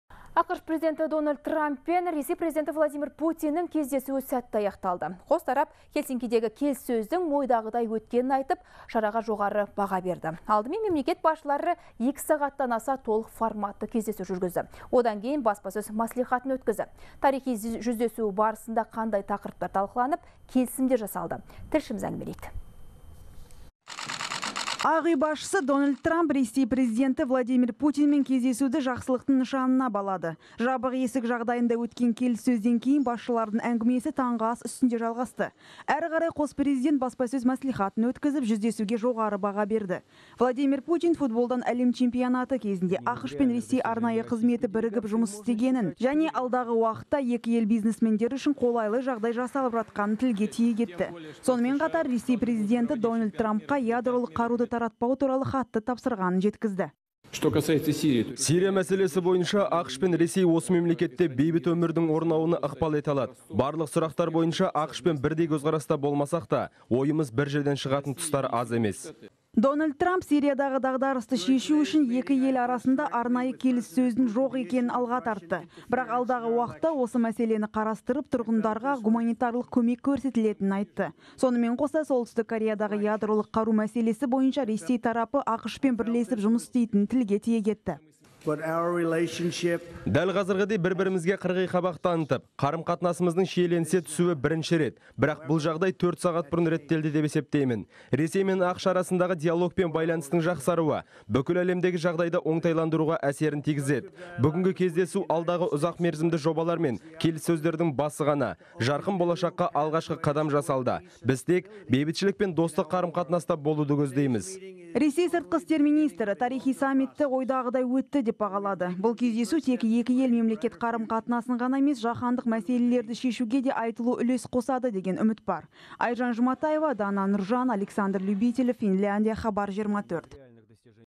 АҚШ пен Ресей президенттері Хельсинкидегі келіссөзге жоғары баға берді
АҚШ президенті Дональд Трамп пен Ресей президенті Владимир Путиннің кездесуі сәтті аяқталды.

Қос тарап Хельсинкидегі келіссөздің ойдағыдай өткенін айтып, шараға жоғары баға берді. Алдымен, мемлекет басшылары 2 сағаттан аса толық форматты кездесу жүргізді. Одан кейін баспасөз мәслихатын өткізді. Тарихи жүздесу барысында қандай тақырыптар талқыланып, келісімдер жасалды?

Ақ үй басшысы Дональд Трамп Ресей президенті Владимир Путинмен кездесуді жақсылықтың нышанына балады. Жабық есік жағдайында өткен келіссөзден кейін басшылардың әңгімесі таңғы ас үстінде жалғасты. Әрі қарай қос президент баспасөз мәслихатын өткізіп, жүздесуге жоғары баға берді. Владимир Путин футболдан әлем чемпионаты кезінде АҚШ пен Ресей арнайы қызметі бірігіп жұмыс істегенін және алдағы уақытта екі ел бизнесмендері үшін қолайлы жағдай жасалып жатқанын тілге тиек етті. Сонымен қатар Ресей президенті Дональд Трампқа «ядролық қаруды таратпау» туралы хатты тапсырғанын жеткізді.